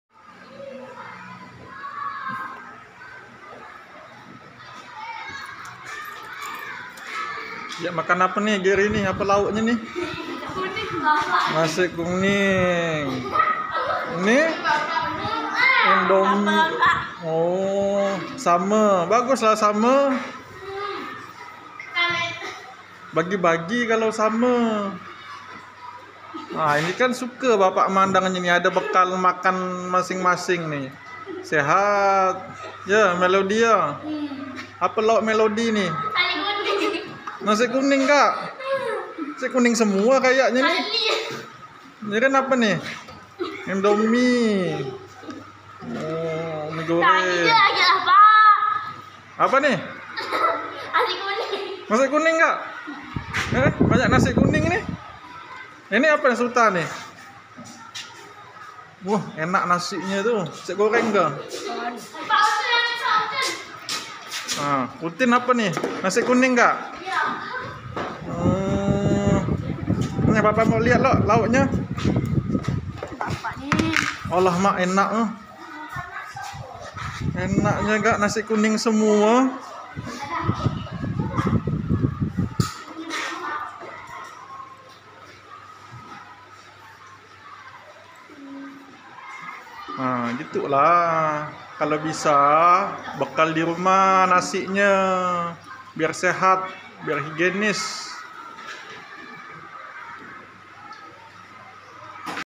Bekal Nasi
Siswa siswi bekal makanan di saat jam istirahat 09.15 WIB